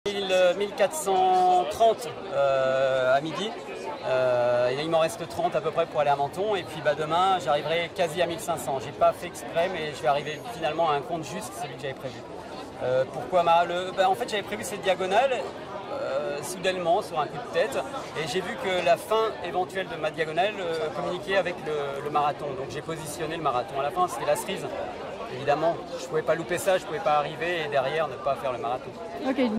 Rencontre avec Guillaume Charbonneau, trailer amateur
Le Dijonnais Guillaume Charbonneau, trailer amateur de 47 ans, est parti du Finistère pour rallier la Côte d'Azur. Un périple de 1500 km qui s'achève avec le marathon de Nice-Cannes ce dimanche 3 novembre et quelques foulées pour aller jusqu'à Menton. Fin de cette folle diagonale.

Intervenant : Guillaume Charbonneau, trailer amateur

Un reportage de : Denise Delahaye et Frédéric Cerulli